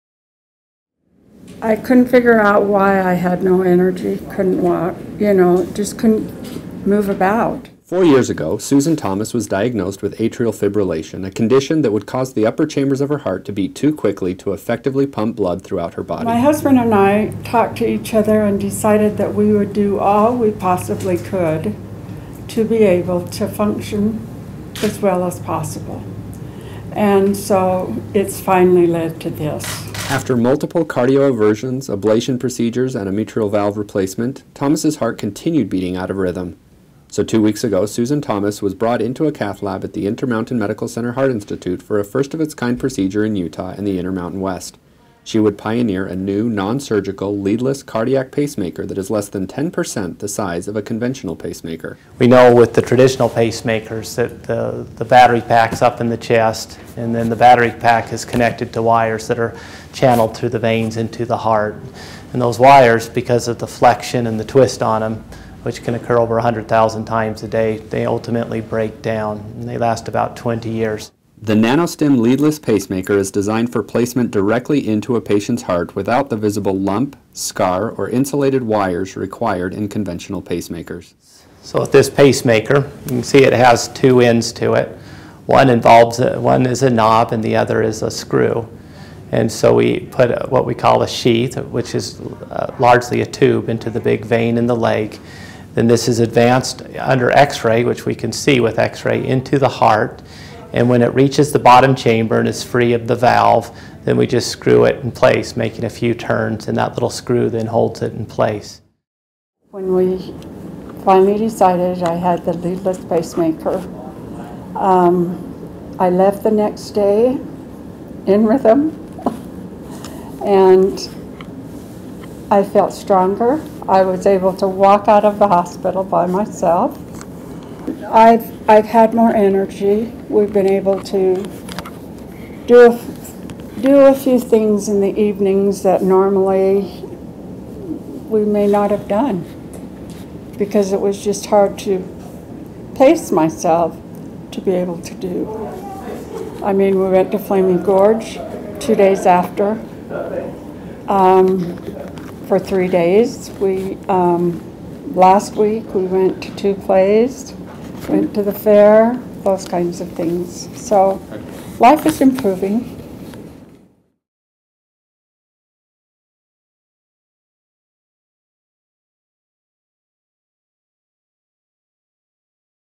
Non-surgical Leadless Cardiac Pacemaker Implanted - A Utah Medical First
A Logan woman makes Utah heart history as cardiologists from the Intermountain Medical Center Heart Institute implant the Intermountain West's first leadless cardiac pacemaker. Find out why the technology is better for patients.